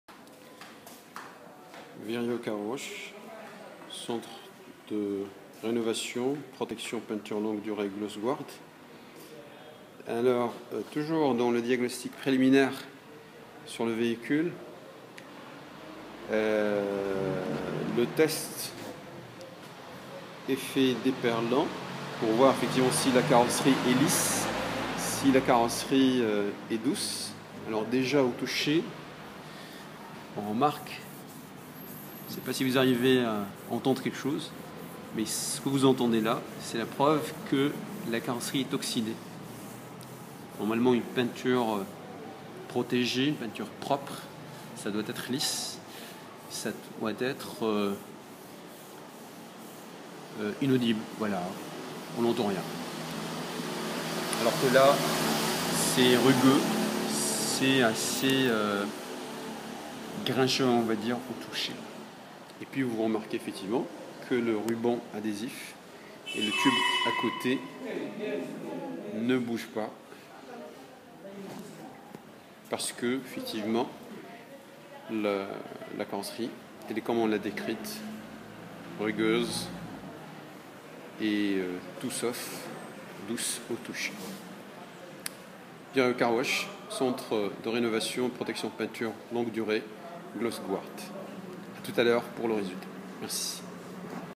L'Avant-Traitement Gloss Guard Paint Protection By VIREO Car Wash (Casablanca)
Le traitement GLOSS GUARD® a des qualités exceptionnelles lui conférant des applications extrêmes dans l'industrie automobile et même aéronautique.

Issue des nanotechnologies, la molécule de Poly Tétra Fluoro Ethylène (P.T.F.E.), commercialement entendue TEFLON®, assure un coefficient de frottement extrêmement faible et une excellente résistance thermique et chimique, pour une protection longue durée des peintures automobiles, contre les pollutions quotidiennes telles que les insectes, le dioxyde de carbone, la résine, ...

Ainsi, l'entretien régulier des véhicules devient plus facile et plus rapide, et la surface carrosserie extrêmement lisse et douce au touché. En résistant aux rayons U.V., la peinture garde sa teinte d'origine et le verni une brillance éclatante.

VIREO™ Car Wash Maroc appartient désormais au réseau mondial des centres de protection peinture longue durée GLOSS GUARD®, l'unique centre agréé en Afrique, basé à Casablanca.